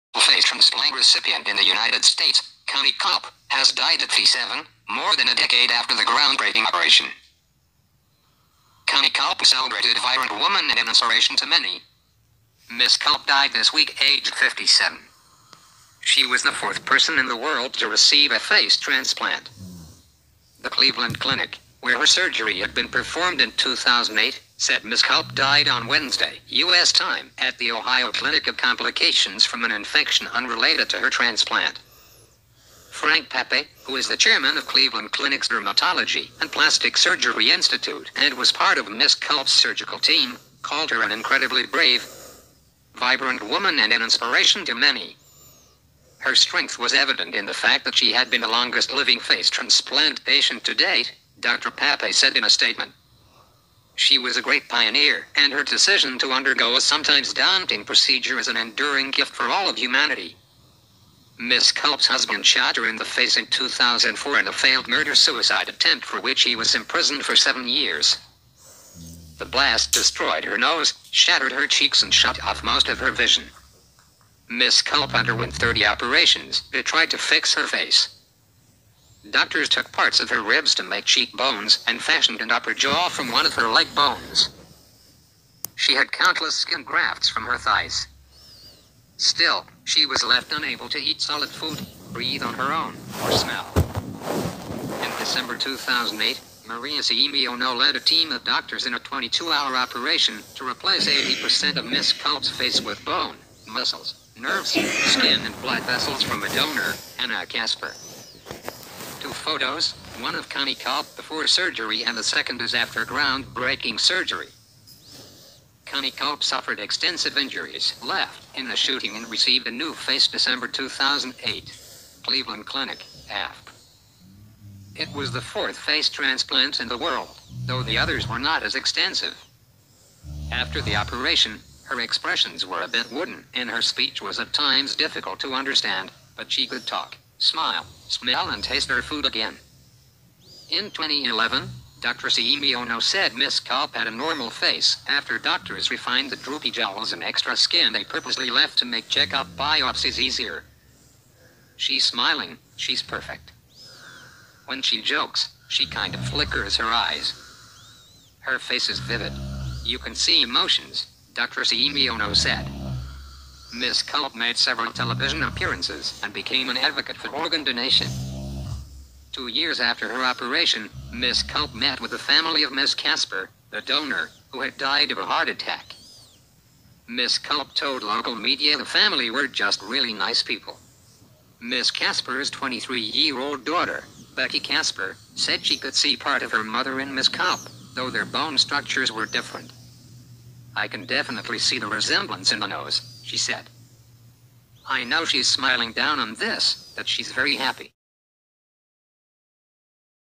Connie Culp, the 1st face transplant recipient in the U.S., dies at 57
The reference for this video is CNN News